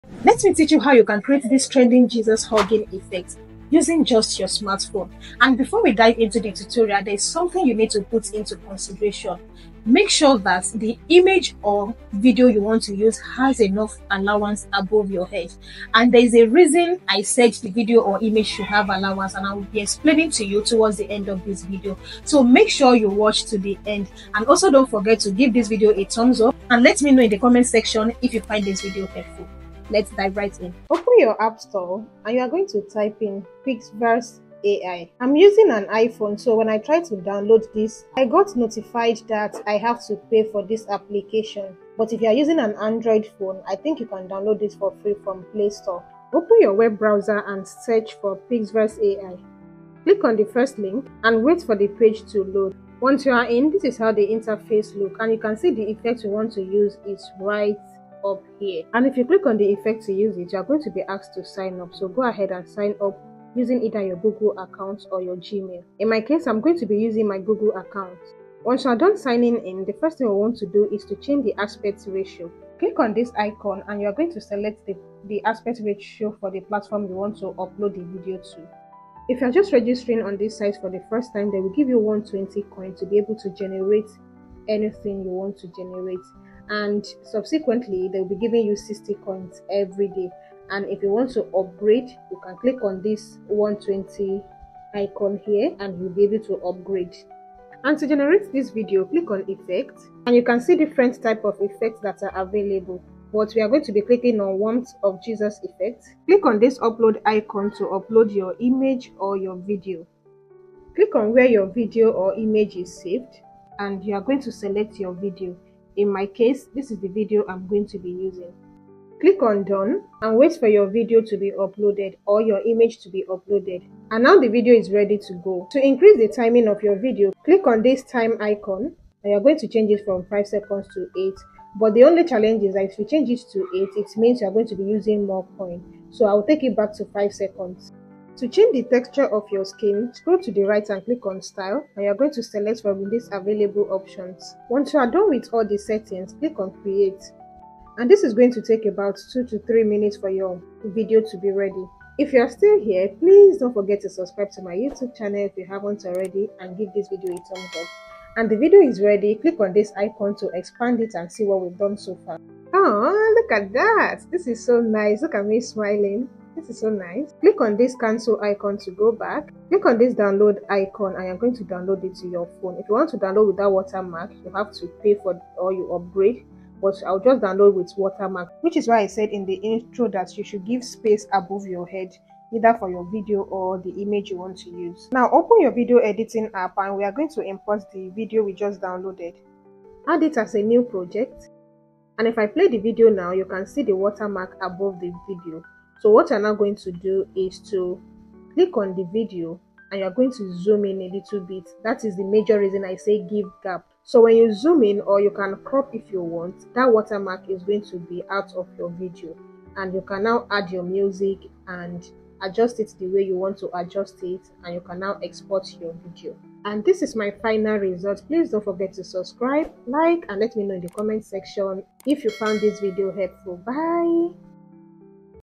How to Use Pixverse AI to Create Jesus Hugging Effect for Free | NO WATERMARK
Want to create the trending Jesus Hugging Effect using Pixverse AI—with no watermark and for free? This step-by-step tutorial will show you how to generate stunning AI-generated images effortlessly! Whether you're a content creator, digital artist, or AI enthusiast, this guide is for you.

What You’ll Learn in This Video:
✅ How to access Pixverse AI and create stunning images
✅ How to apply the Jesus Hugging Effect step by step
✅ How to save your image without a watermark